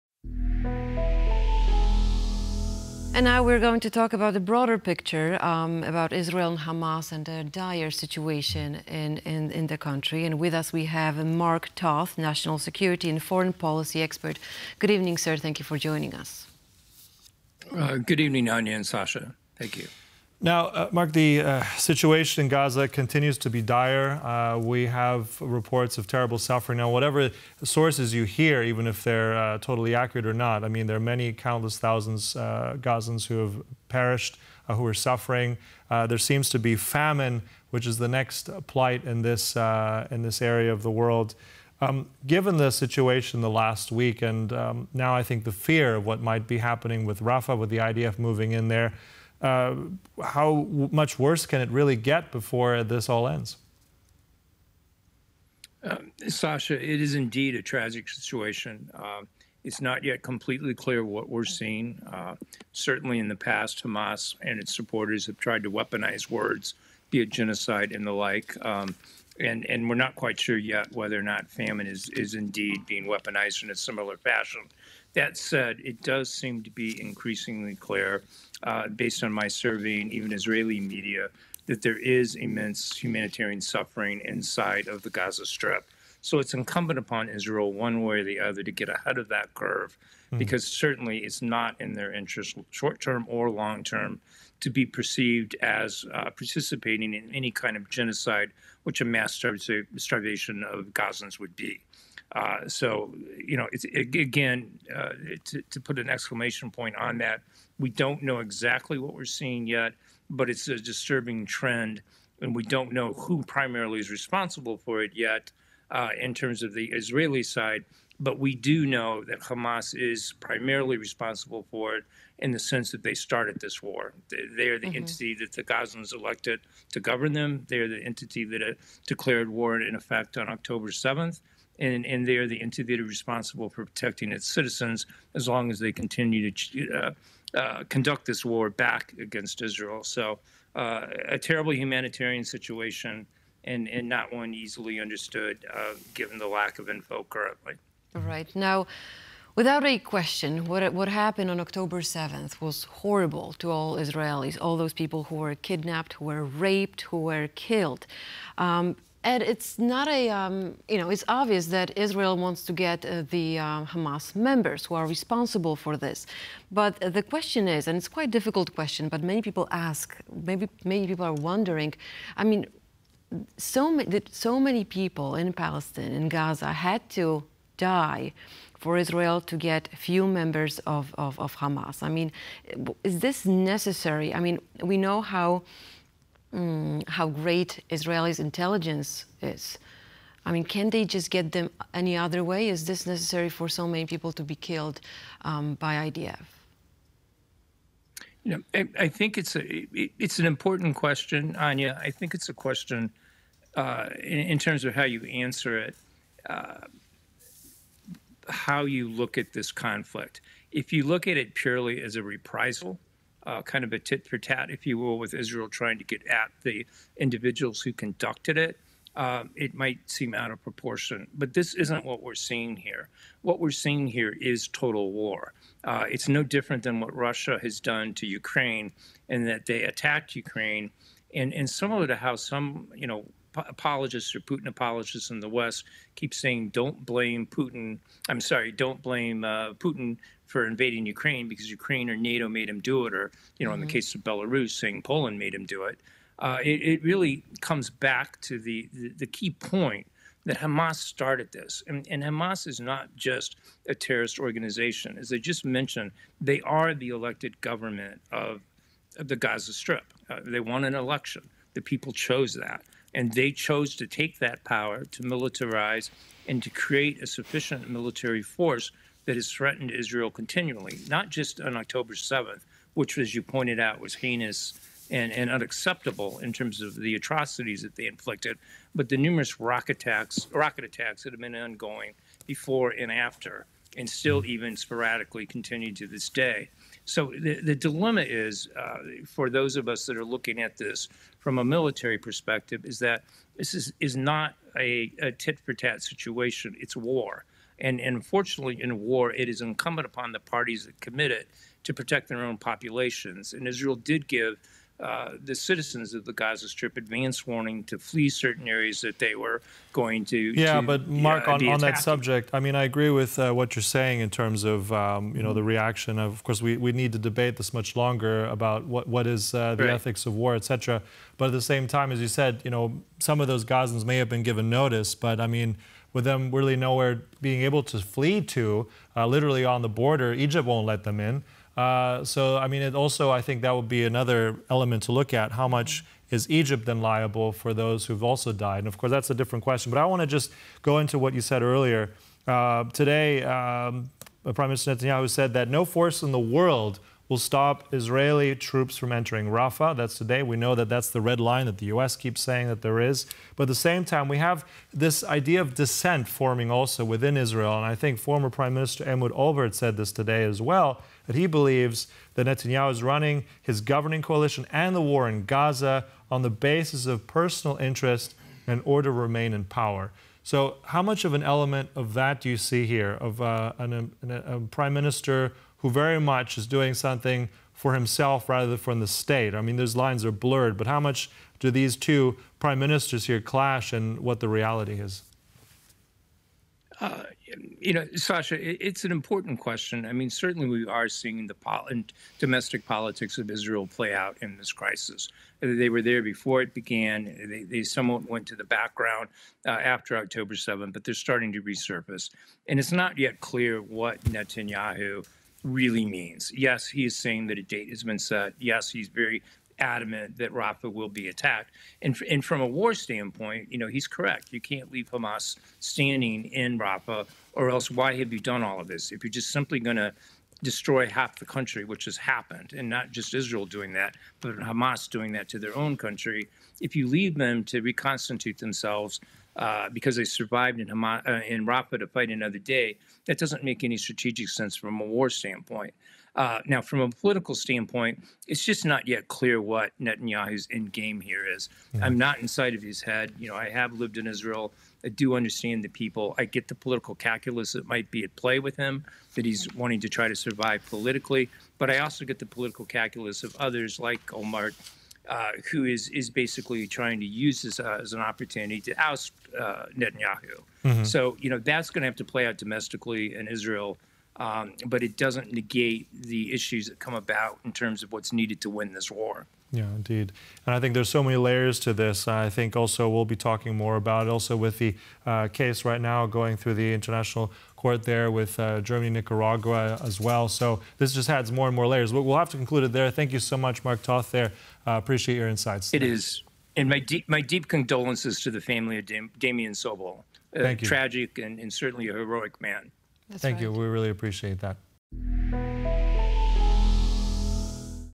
Benjamin Netanyahu sets a date for Rafah invasion | Mark Toth
Israeli Prime Minister Benjamin Netanyahu has announced that a specific date has been set for a ground offensive in the southern Gaza city of Rafah. This decision comes amidst ongoing truce talks with Hamas in Cairo, although a conclusive agreement seems distant. To tell us more we invited foreign policy expert Mark Toth.